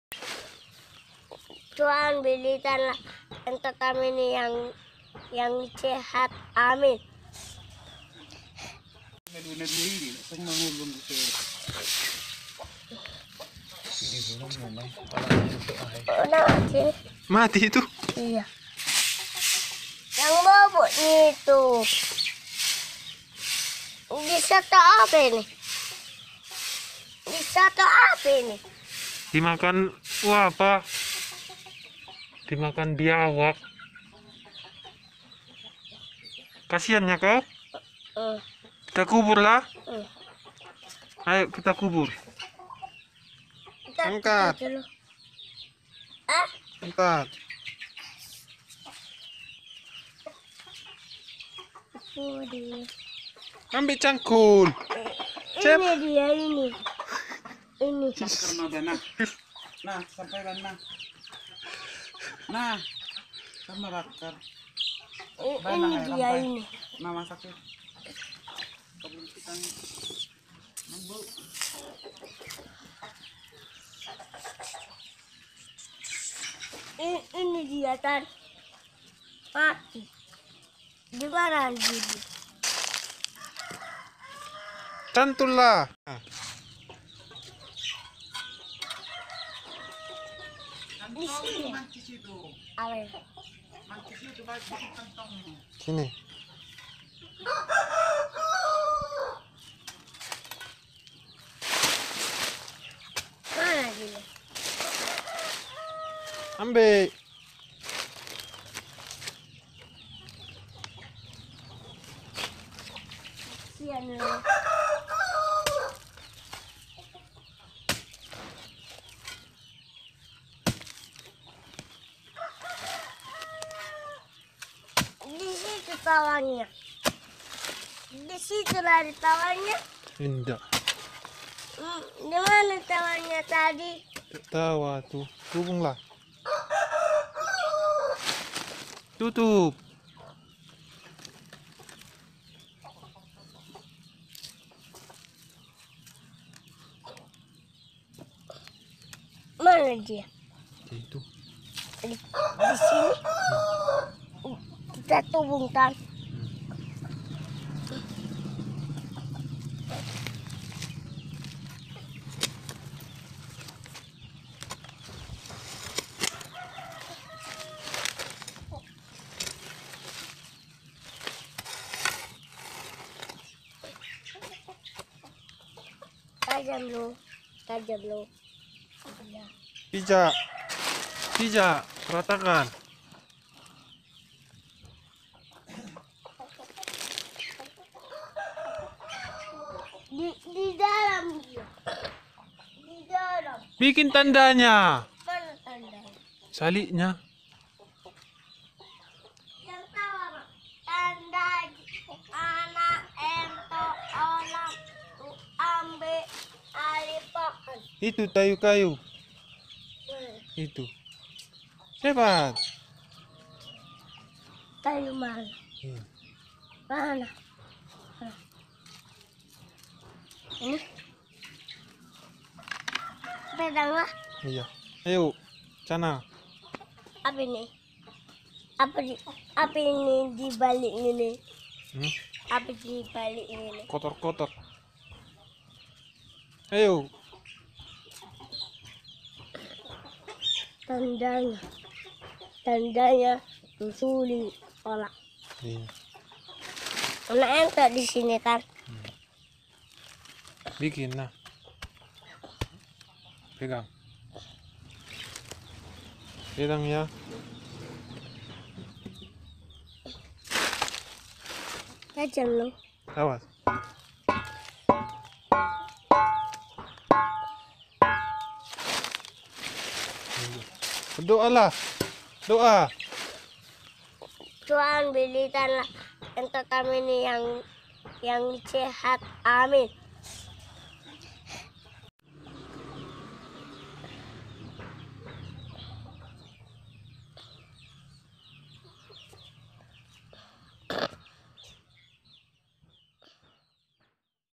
Sedih! Doa Anak Untuk Hewan Peliharaan yang Mati.
Doa anak Kecil Untuk Hewan Peliharaan kesayangan yang mati.